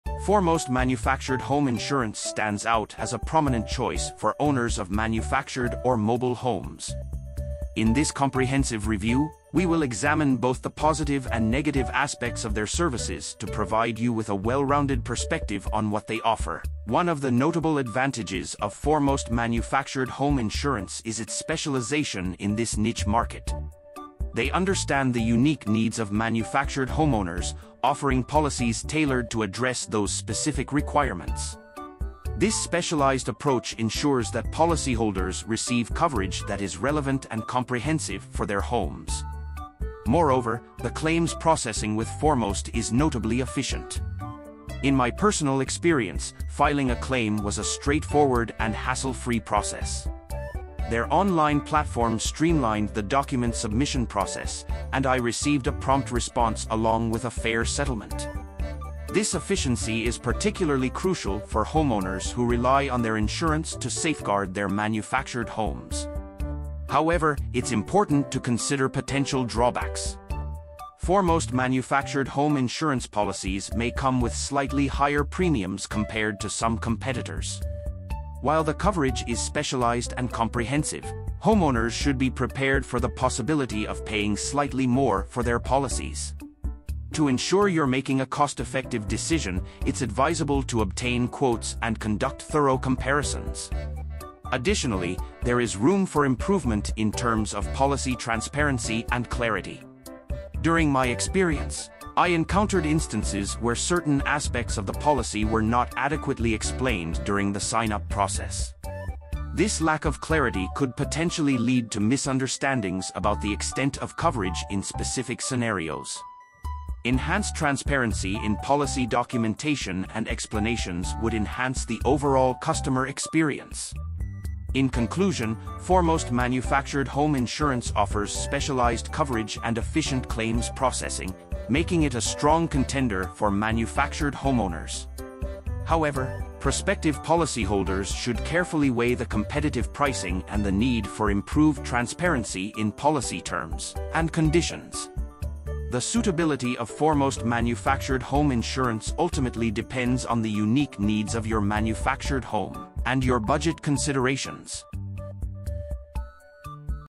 🔥 Foremost Manufactured Home Insurance Review: Pros and Cons
Foremost Manufactured Home Insurance is a reputable provider dedicated to serving the specific needs of manufactured or mobile homeowners. In this comprehensive review, we will delve into both the positive and negative aspects of their insurance offerings, providing you with valuable insights to make an informed decision about protecting your manufactured home.

One of the standout advantages of choosing Foremost Manufactured Home Insurance is their specialization in this niche market. They have a deep understanding of the unique requirements of manufactured homeowners, offering policies tailored to provide comprehensive coverage. This specialized approach ensures that policyholders receive protection that is specifically designed to safeguard their homes, which can be quite distinct from traditional houses.

Furthermore, Foremost is known for its efficient claims processing. In my personal experience, filing a claim with them was a seamless and hassle-free process. Their user-friendly online platform made it easy to submit the necessary documents, and I received a prompt response along with a fair settlement. This efficiency is especially important for manufactured homeowners who depend on their insurance to safeguard their valuable assets.

However, it's essential to consider potential drawbacks as well. Foremost Manufactured Home Insurance policies may come with slightly higher premiums compared to some competitors. While the coverage is specialized and comprehensive, homeowners should be prepared for the possibility of paying slightly more for their policies. To ensure that you're getting the best value for your investment, it's advisable to obtain quotes and conduct thorough comparisons with other insurance providers.

Moreover, there is room for improvement in terms of policy transparency and clarity. During my experience, I encountered instances where certain aspects of the policy were not adequately explained during the sign-up process. This lack of clarity could potentially lead to misunderstandings about the extent of coverage in specific scenarios. Enhanced transparency in policy documentation and explanations would undoubtedly enhance the overall customer experience.

In conclusion, Foremost Manufactured Home Insurance stands as a specialized provider with efficient claims processing, making it a solid choice for manufactured homeowners. The suitability of Foremost Manufactured Home Insurance ultimately depends on the unique needs of your manufactured home and your budget considerations.